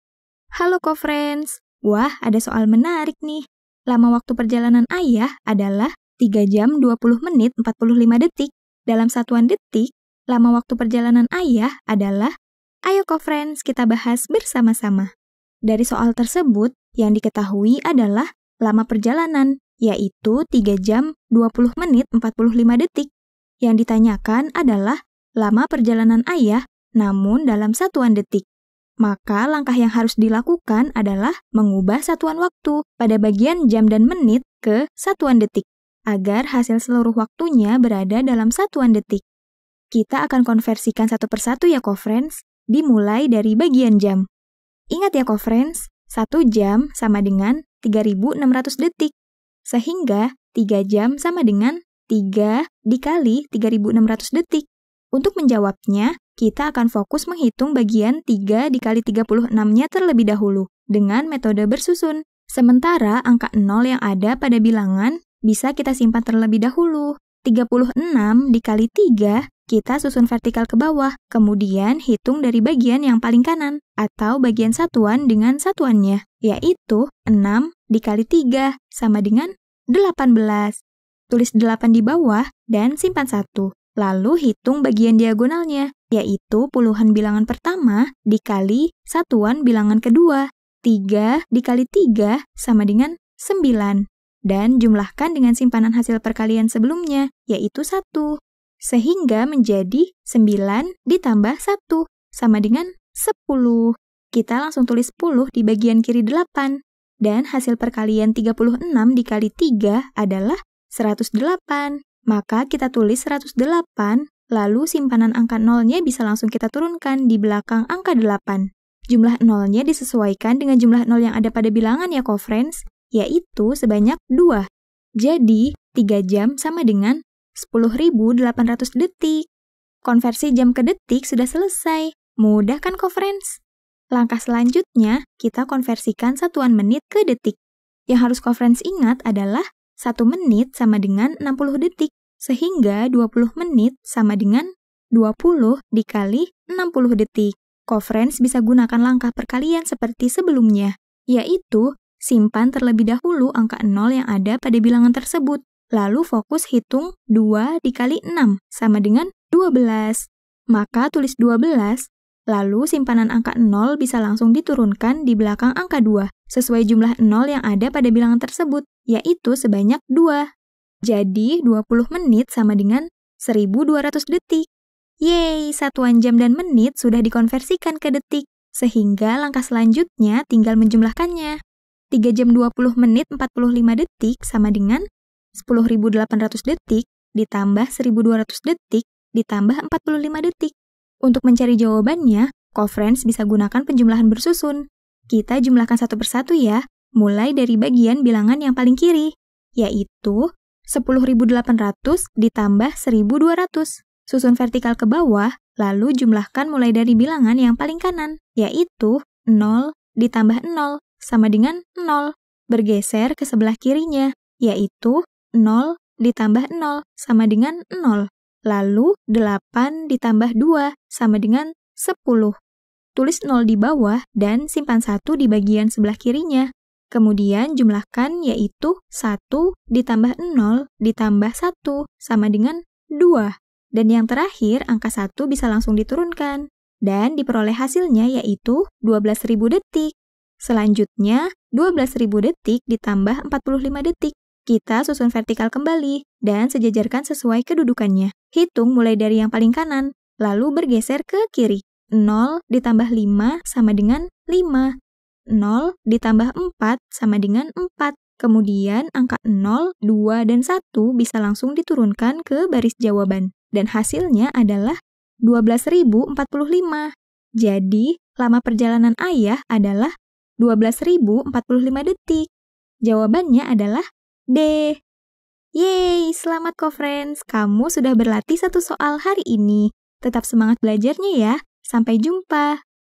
Larrı waklu perjalanan ayah adalah 3 jam 20 menit 45 detik. Dalam satuan detik, lama perjalanan a...
Larrı waklu perjalanan ayah adalah 3 jam 20 menit 45 detik. Dalam satuan detik, lama perjalanan ayah adalah ... detik.

Topik atau Materi: Konversi satuan Waktu - Kecepatan dan Debit - Aritmatika - Matematika Kelas 5 SD - KTSP

Sumber Soal: AMPUH Matematika Kelas 5 Semester 1, KTSP, HYT, 2009 , Esis

00:00 : Bedah Soal
01:00 : Konsep, rumus dan pengertian Konversi satuan Waktu
01:18 : Langkah penyelesaian soal
03:43 : Jawaban akhir
04:13 : Penutup